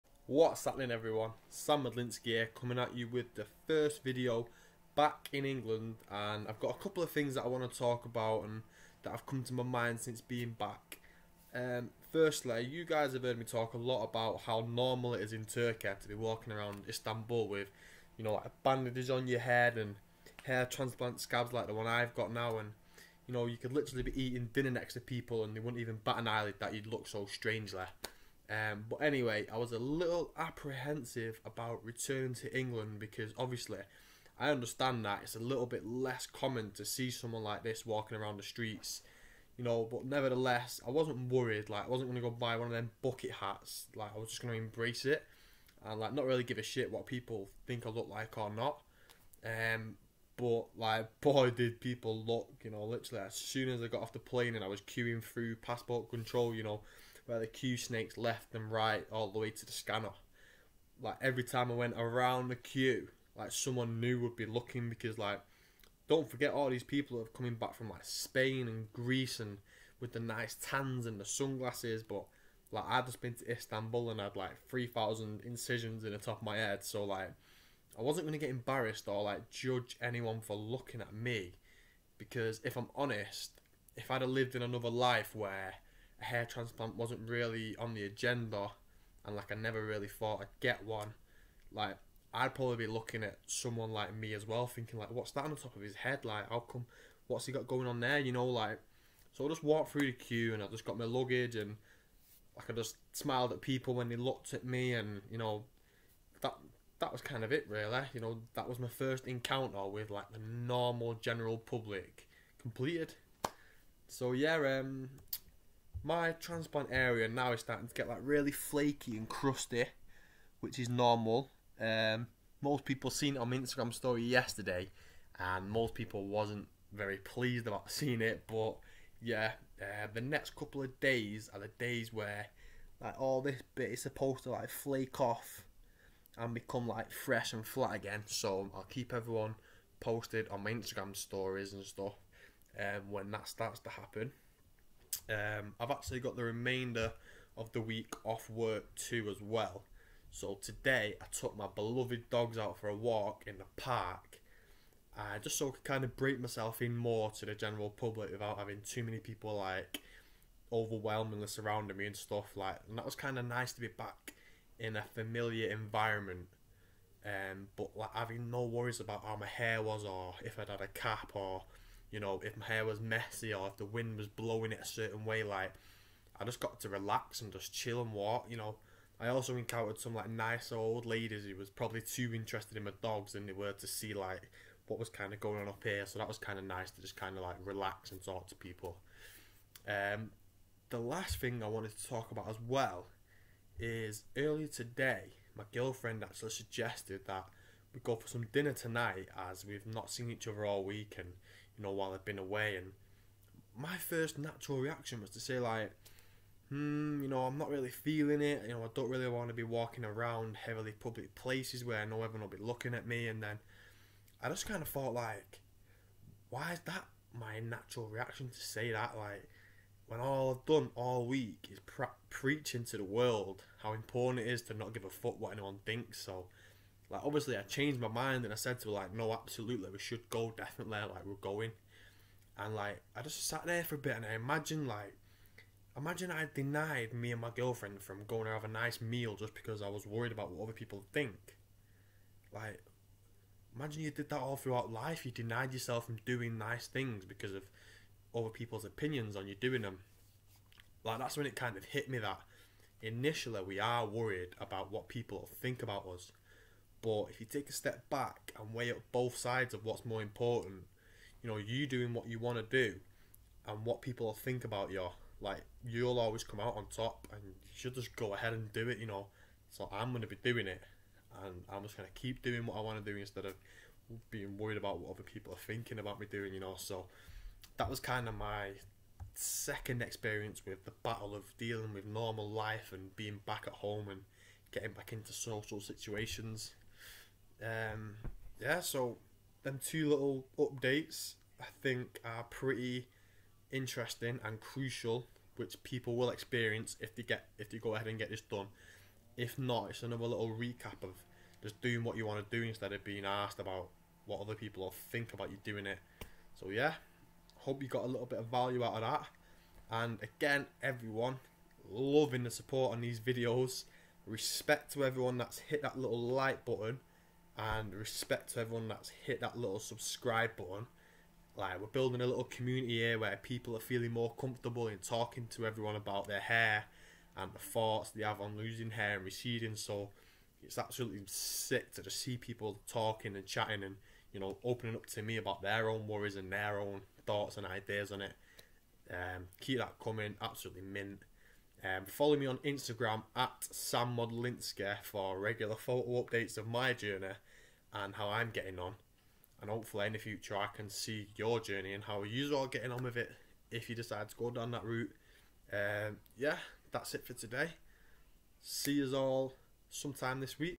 DAY 7 - HAIR TRANSPLANT REACTION - I'M HOME NOW !
Whats happening everyone!

Just wanted to talk about my personal experience after returning from Istanbul with my newly transplanted hair, and as soon as I got off the plane there was people looking and interested in what was going on on top of my head!

Which i completely expected you know, it's not a common thing to see in the UK, so I was mentally prepared for people looking at my hair, so it didn't phase me as much as I thought.

I also almost denied me and my girlfriend from going out for a nice meal as I was initially conscious about what people might think of the scabs on my head.

Luckily I had a reality check of the life i'm trying to build and remembered that me doing what I want is much more important that caring about other peoples opinions!

UPDATE: Check out my video from 25th April for the new products I’m using...

From a company called Watermans, Check the products out on amazon!

- Shampoo & Conditioner

- Serum/Elixir Minoxidil Substitute

- GrowPro Hair Vitamins

So follow me on instagram @SamModlinsky to see the next couple of days when my FUE Hair Transplant is supposed to enter the first stage when these scabs flake away and begin new growth!